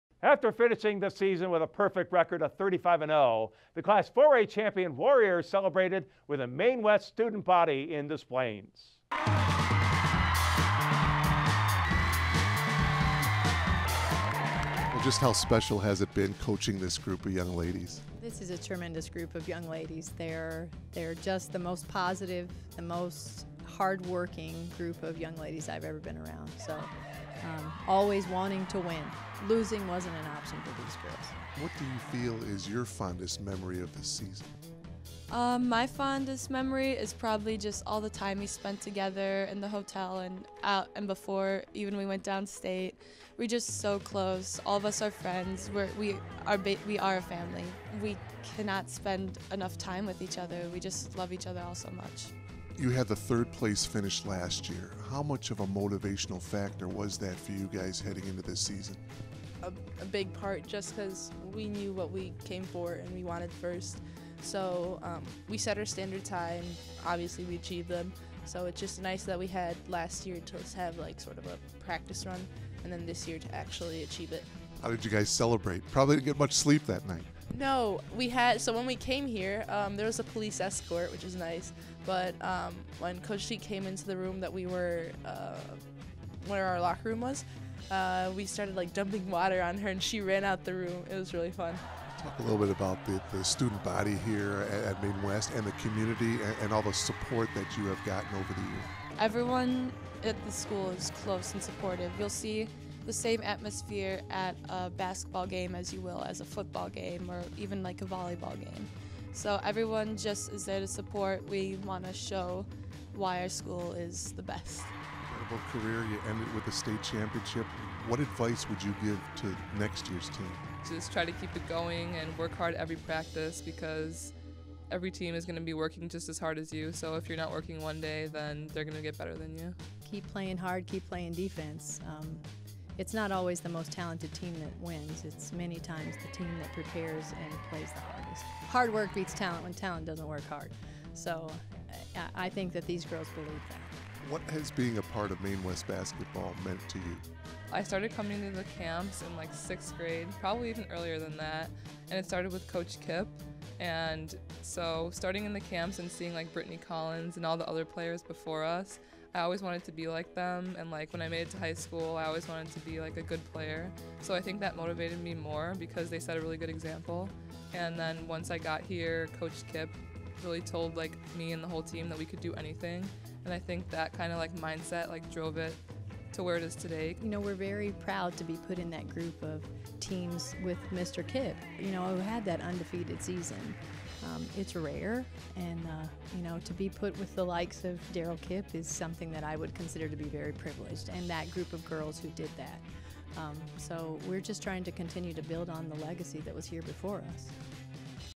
Maine West 4A Championship Celebration
Maine West celebrates their 2019 Girls Basketball Class 4A State Championship in Des Plaines